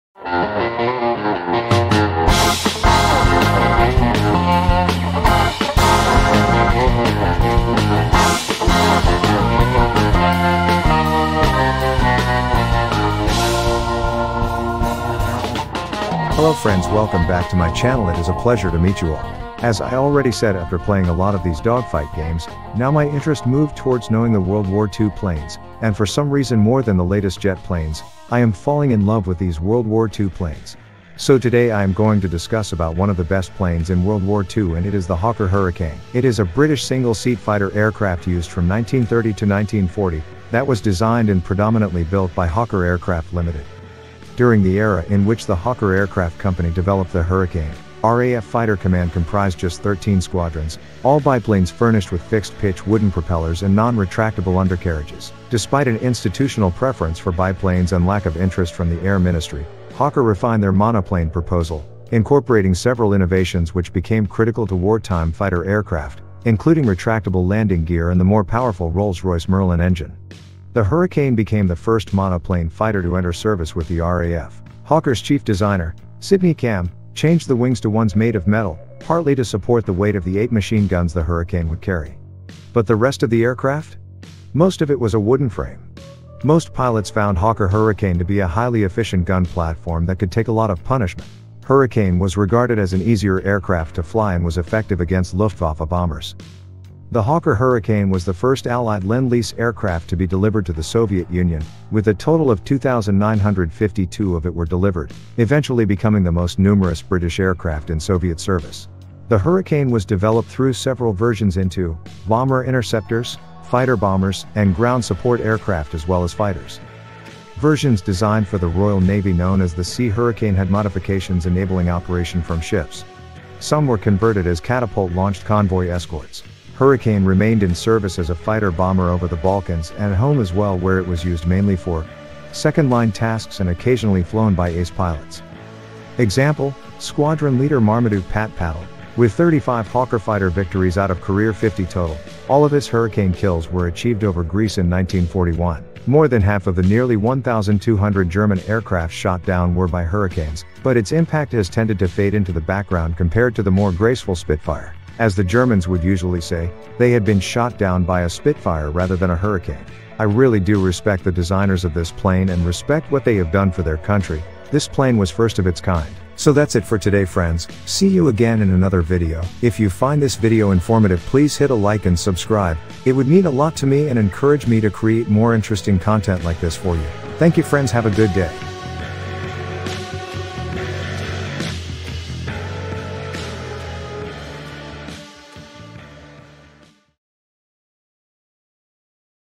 The First Monoplane Fighter | Hawker Hurricane Plane details | lessons of war
The Hurricane became the first monoplane fighter to enter service with the RAF.
Hawker’s chief designer, Sidney Camm, changed the wings to ones made of metal, partly to support the weight of the eight machine guns the Hurricane would carry. But the rest of the aircraft was made of wooden frames. most pilots found Hawker Hurricane to be a highly efficient gun platform that could take a lot of punishment.

Fighter aircraft of the 1930s–40s
Retractable landing gear.
Engine - Rolls-Royce Merlin.
Support the weight of eight machine guns.
ACE- Marmaduke "Pat" Pattle,  35 KILLS
Nearly 1,200 German aircraft were shot down

Which plane shot down the most planes in WW2